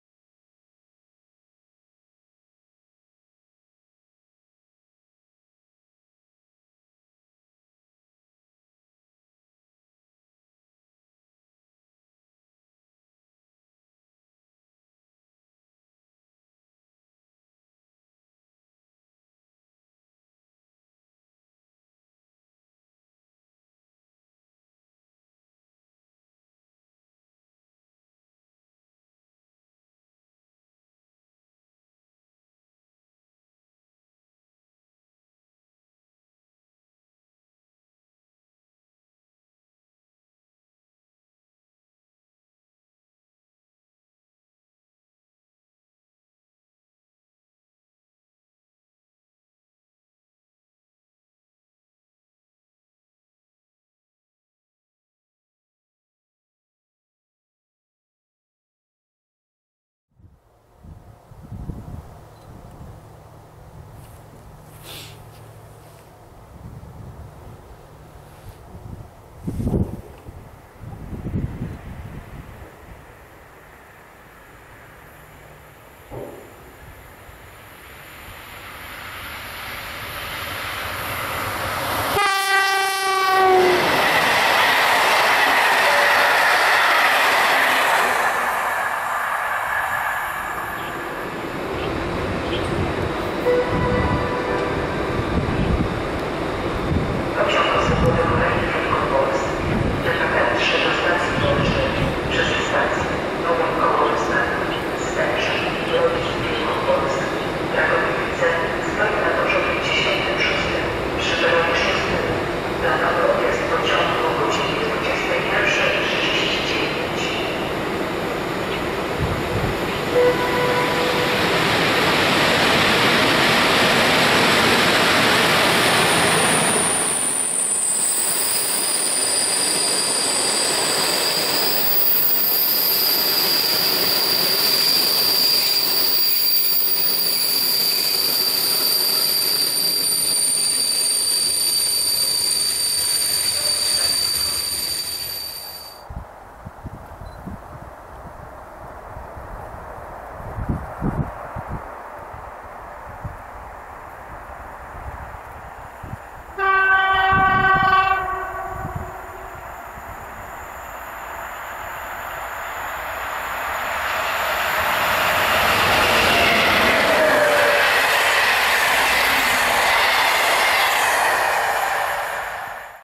[PKP Intercity] "Eksperyment Słowacja" na TLK Bachus & Lednica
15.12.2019 wszedł nowy rozkład jazdy. Pojawiło się w nim nowe międzynarodowe połączenie TLK "Rozewie", które ze stacji Bielsko Biała Główna przez Zwardoń zostało wydłużone na Słowację obsługując stację Cadca i kończąc bieg na stacji Zilina.

Z racji kolejnych braków taborowych, przewoźnik PKP Intercity zdecydował się na wynajem słowackich wagonów do częściowej obsługi pociągu. Jednak wypożyczonych zostało dużo więcej wagonów, które można spotkać na dwóch innych, w pełni krajowych relacjach. Są to pociągi TLK "Bachus" relacji Zielona Góra - Gdynia oraz TLK "Lednica" relacji Gdynia - Poznań.

Zapraszam na film będący prezentacją trzech przejazdów wymienionych pociągów. Jak widać po filmach / zdjęciach zamieszczonych w filmiku standard wagonów znacząco odbiega od tych widocznych na wielu krajowych połączeniach (zmodernizowanych czy to w Pesie czy też w FPS).
Można zauważyć, że część wagonów słowackich ma też świeżo przeprowadzone rewizje, gdyż (zgodnie z informacjami medialnymi, PKP Intercity wykazywało chęć oszczędności przy wynajmnie wagonów. Strona słowacka chciała zaproponować wagony o wyższym standardzie, niż te które obecnie zostały skierowane do Polski).